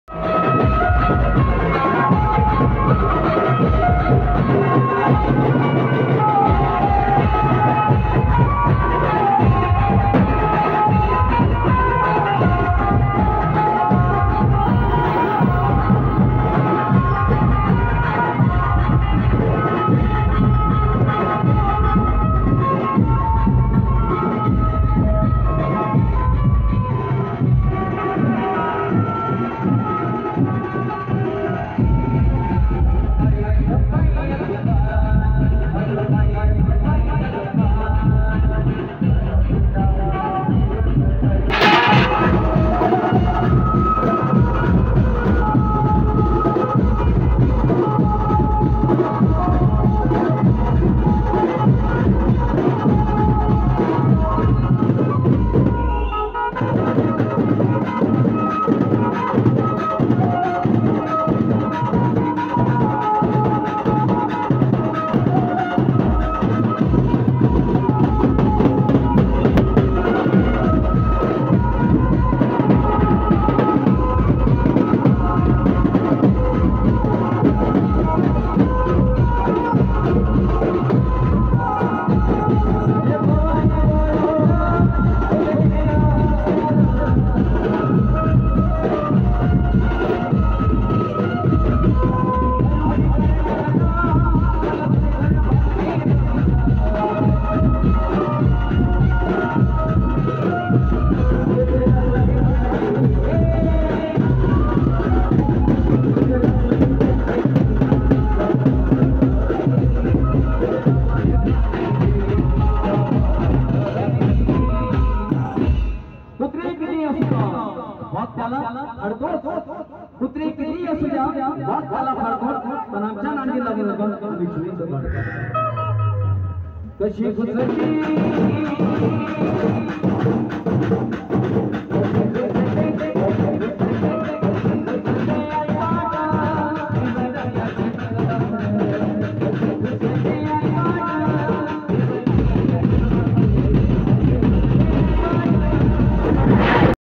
New Famous Band Activepad Mix Song | न्यू फेमस बँड पार्टी अकोला
Official 🕉 Dhingana

Use Headphone For Best Sound Quality

About Video :
Title - New Famous Band Activpad Mix
Band - New Famous
Song - Activepad Mix
Instrument -Octapad, Banjo
Code - Blue White Lining